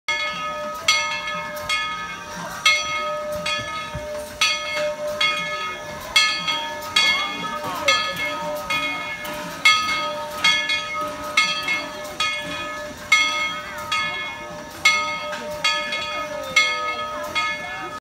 Thomas the Tank Engine Train
Watch as Thomas the Tank Engine is ready to leave town.  be careful the train whistles are loud... Video footage recorded in 2013 at Tweetsie Railroad in Blowing Rock, North Carolina..